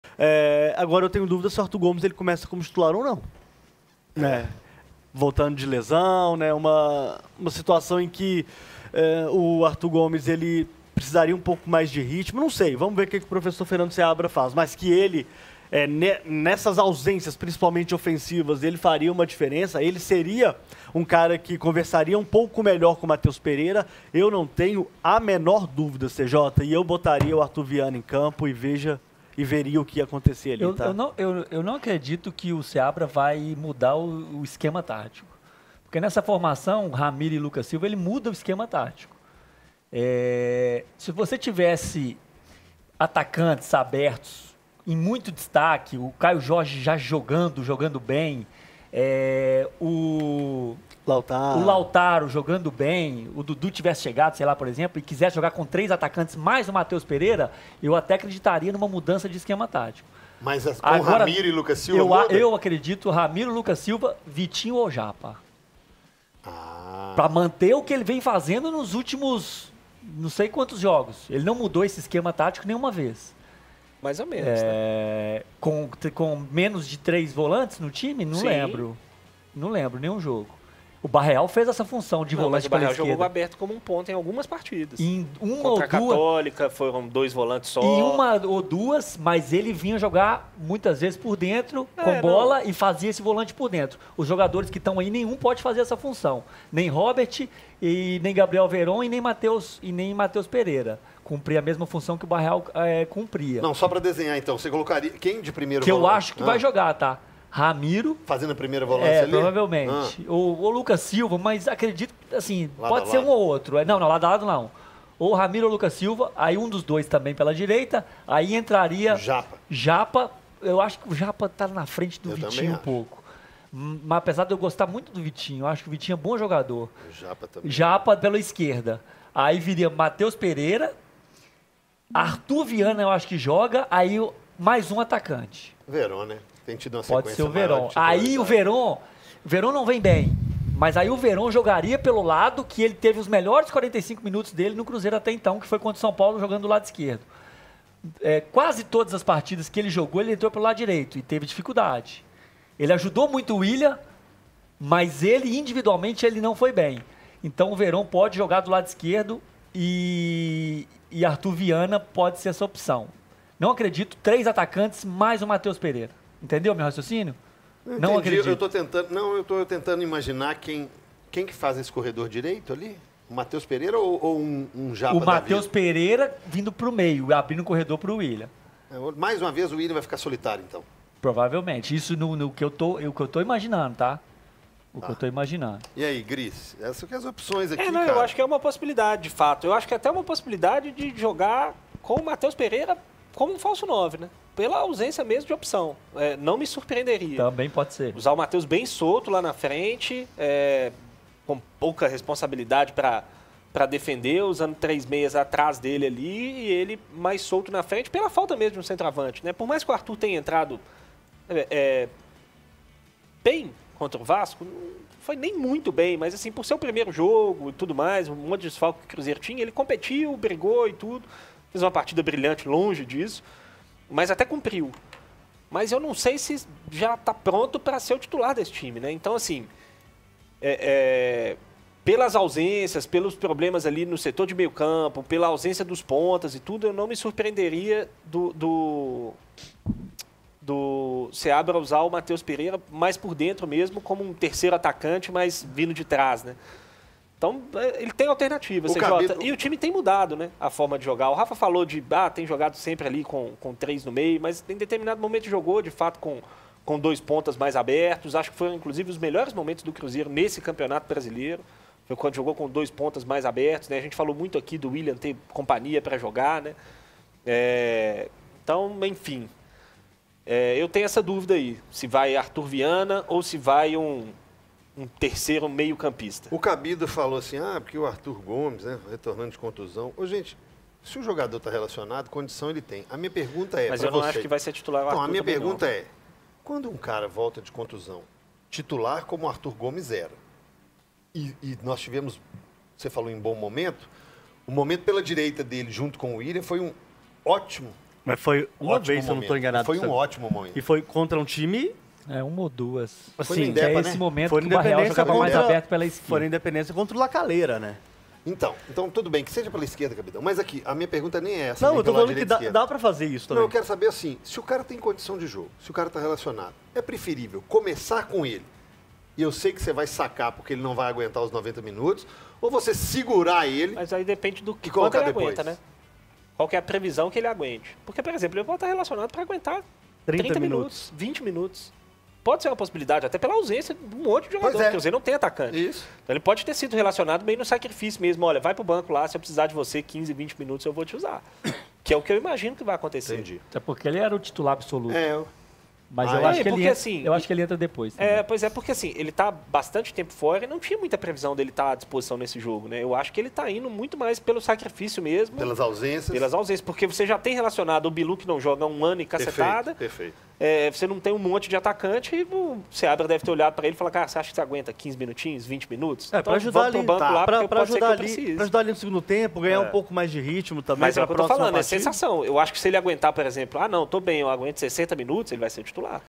ARTHUR GOMES X ARTHUR VIANA: quem tem que começar JOGANDO?
O 98 Esportes debateu sobre quem deve começar jogando no ataque do Cruzeiro, contra o Fluminense. Arthur Gomes, recuperado de lesão, voltará a ser relacionado e vai disputar a vaga com seu xará, o menino Arthur Viana. Quem será que pega a vaga de titular no ataque da Raposa?